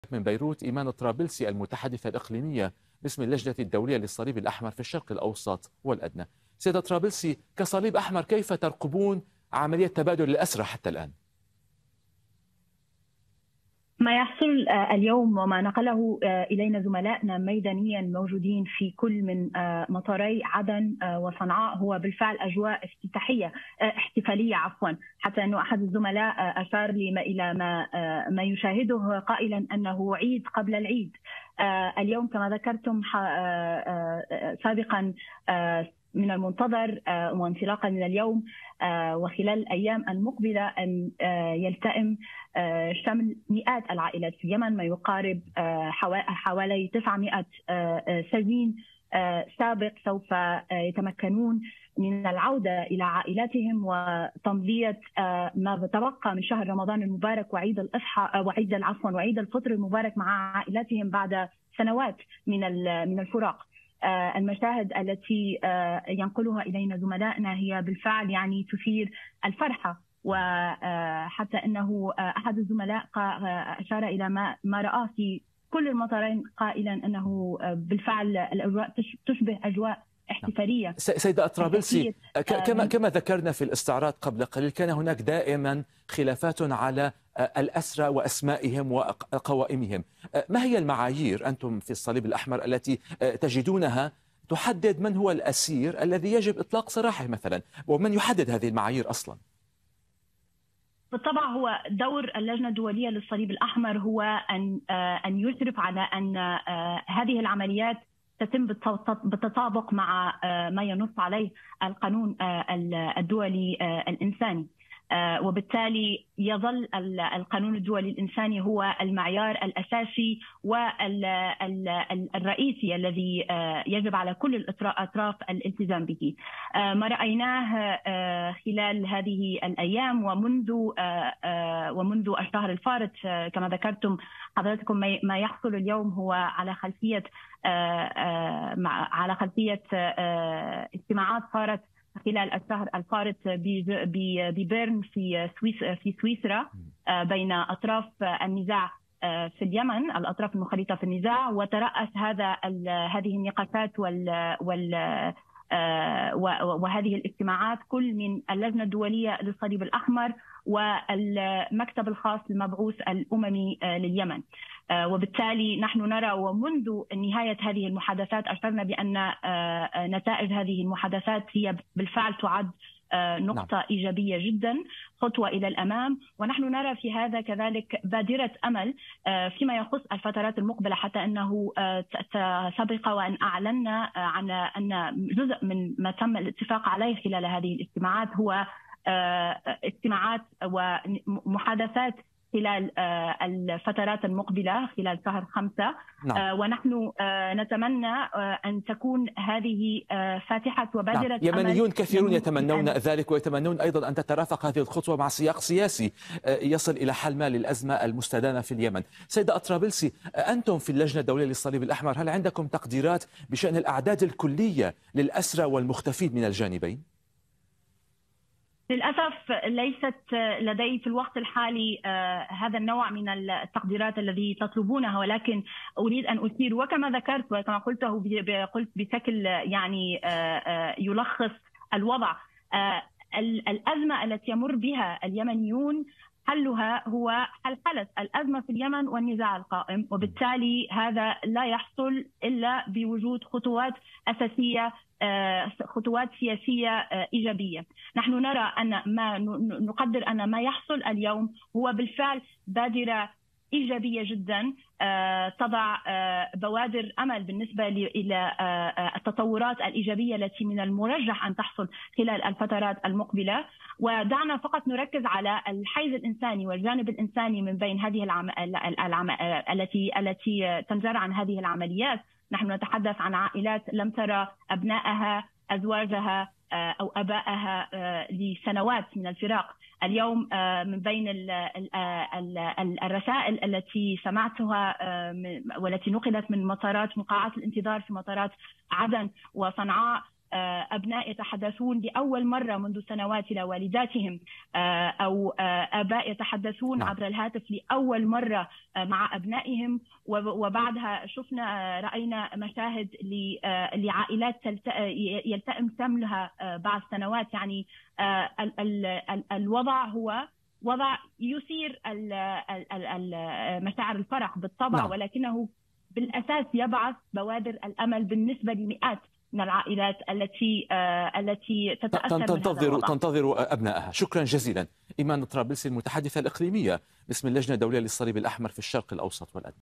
المتحدثة باسم الصليب الأحمر في الشرق الأوسط تعلق على أكبر عملية تبادل للأسرى في اليمن منذ سنوات
أعلنت اللجنة الدولية للصليب الأحمر بدء تنفيذ عملية تبادل الأسرى بين الحكومة اليمنية والحوثيين، في ثاني أكبر عملية تبادل بين التحالف الحكومي وجماعة الحوثيين منذ اندلاع النزاع الدامي في البلاد قبل 8 سنوات.